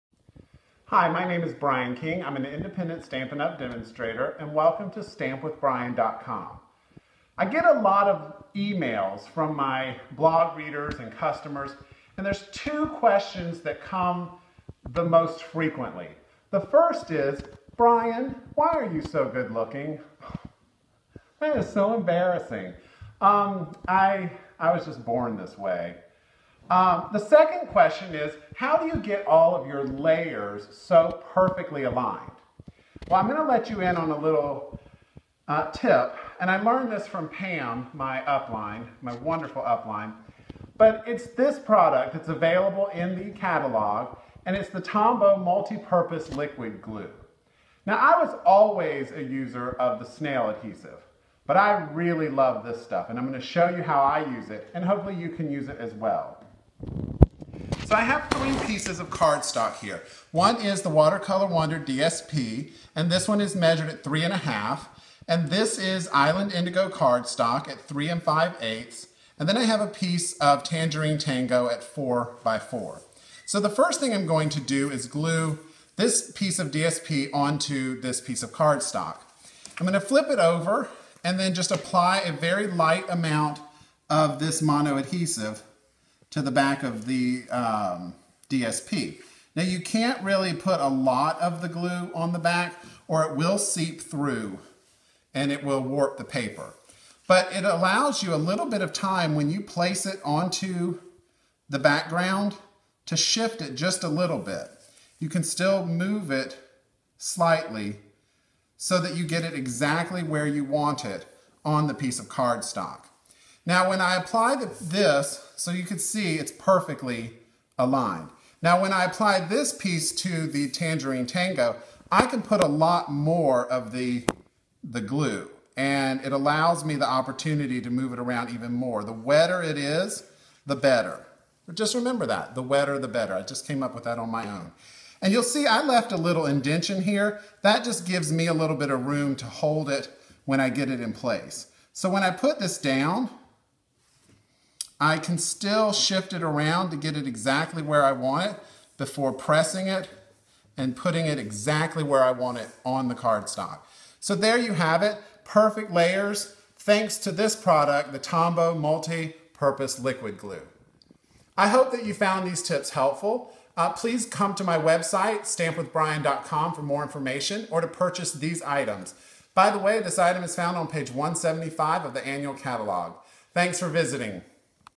Secrets of Alignment on Stampin' Up! greeting cards
Brian King, Independent Stampin' Up! Demonstrator, shares his secrets for perfectly aligning layers of card stock when constructing greeting cards.  For more information about this topic, please go